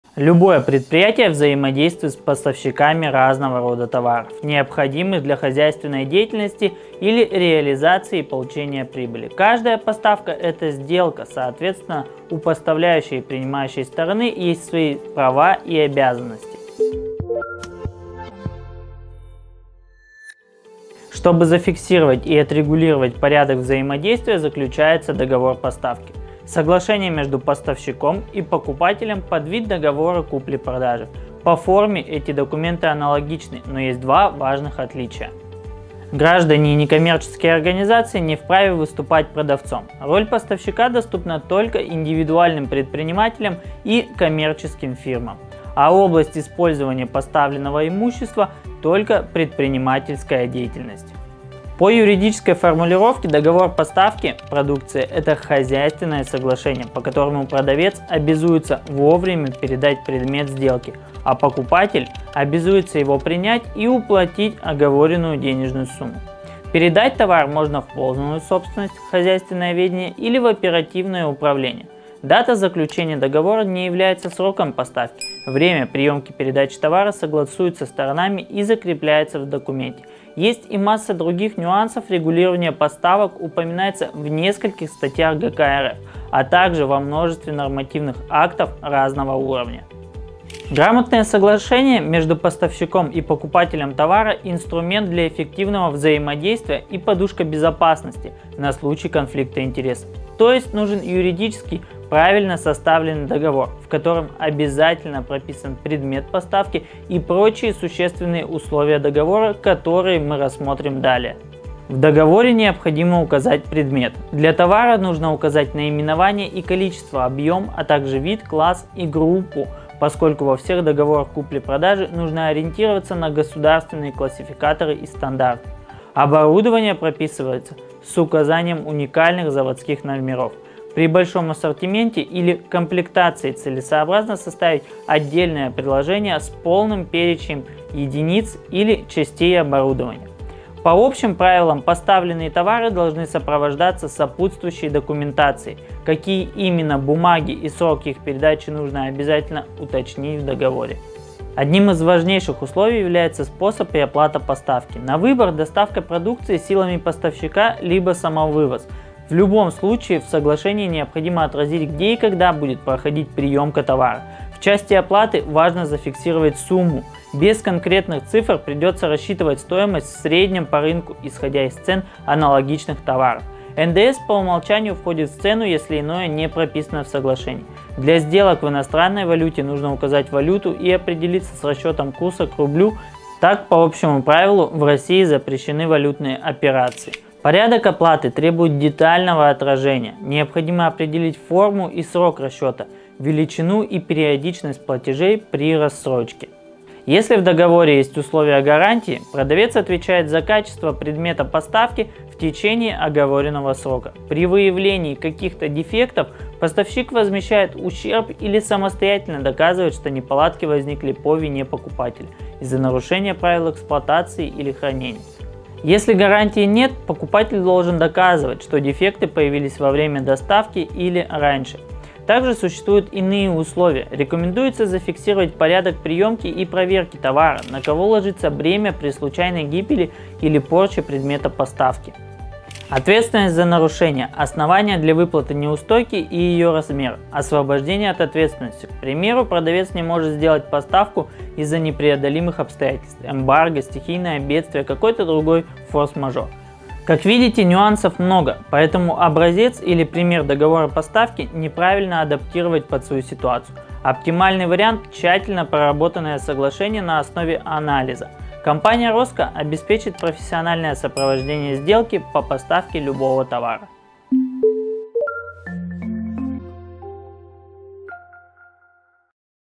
Договор поставки | Консультация юриста
Любое предприятие взаимодействует с поставщиками разного рода товаров, необходимых для хозяйственной деятельности или реализации и получения прибыли.

Каждая поставка – это сделка, соответственно у поставляющей и принимающей стороны есть права и обязанности. Чтобы зафиксировать и отрегулировать порядок взаимодействия заключается договор поставки.

Соглашение между поставщиком и покупателем – подвид договора купли-продажи. По форме эти документы аналогичны, но есть два важных отличия.

Договор поставки продукции – это хозяйственное соглашение, по которому продавец обязуется вовремя передать предмет сделки, а покупатель обязуется его принять и уплатить оговоренную денежную сумму.

Далее Мы рассмотрим:

1. Предмет договора поставки.
2. Способ и оплата поставки.
3. Гарантии.
4. Ответственность за нарушения договора поставки.

Материал подготовлен юрисконсультом "РосКо - Консалтинг и аудит"  Валерием Ивановым.